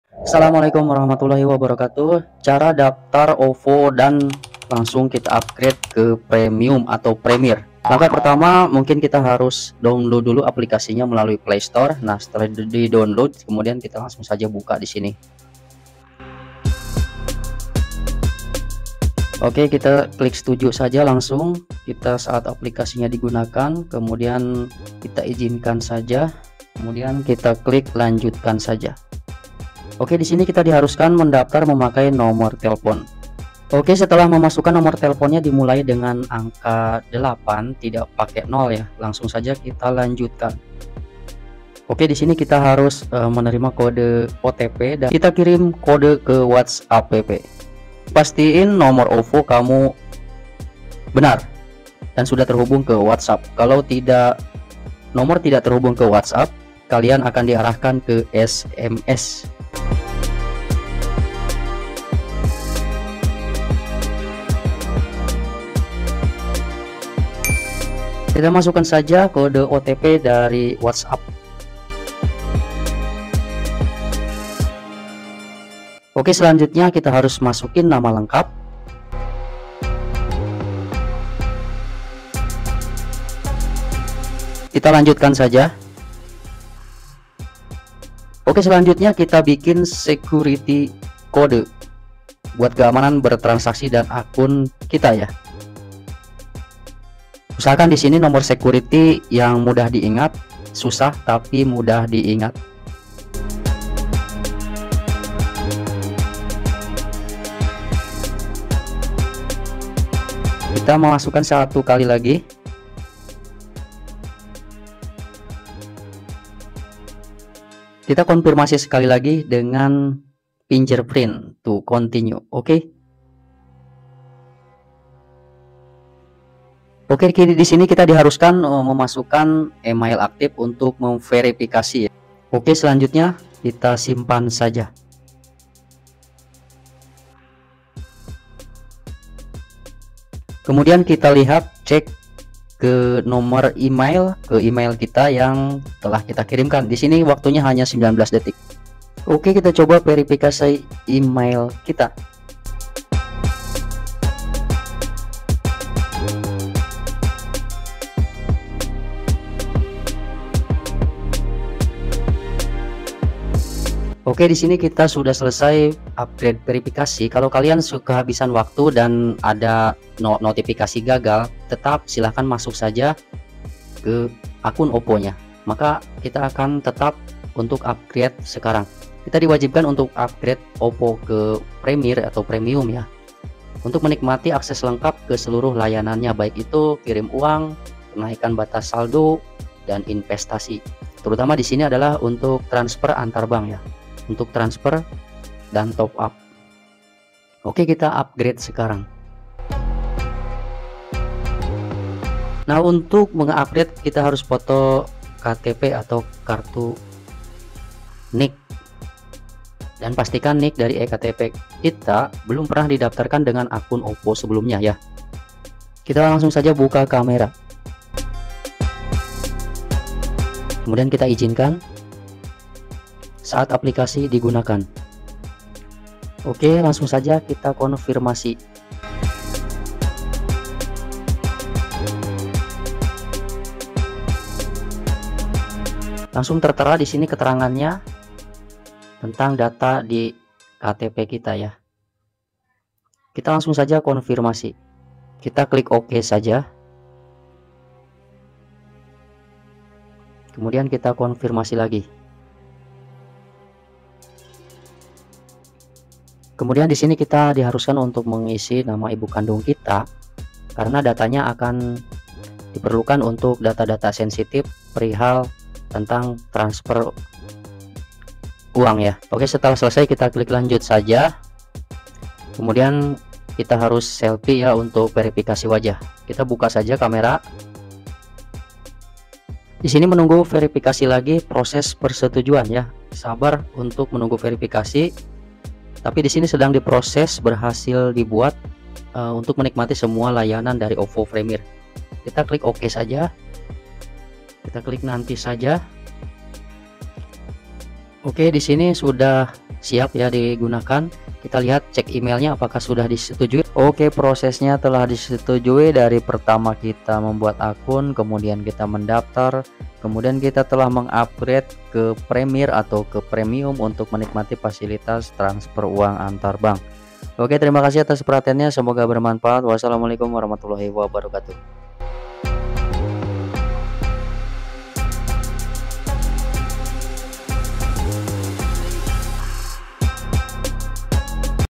Cara daftar ovo langsung upgrade ke premium
Untuk membuat akun dan daftar ovo yang bisa di upgrade ke premium caranya yang pertama kita harus punya aplikasi ovo yang bisa di download atau di instal dari play store

Setelah di download buka saja aplikasinya maka kita di haruskan untuk mendaftar dengan memakai telephon yang masih aktif memverifikasi email aktif dan menulis nama lengkap setelah itu kita harus membuat security code sebagai pengaman akun

Setelah email berhasil di verifikasi maka selanjutnya kita harus meng upgrade ovo ke premier atau premium supaya ovo kita bisa melakukan transfer antar bank, top up dan melakukan tarik tunai di Alfamart dan Indomaret

Langkah upgrade ke ovo premium syaratnya kita harus foto selfie dan mengirimkan foto KTP dan NIK KTP dan mengisi formulir seperti alamat lengkap sesuai KTP dan nama ibu kandung kita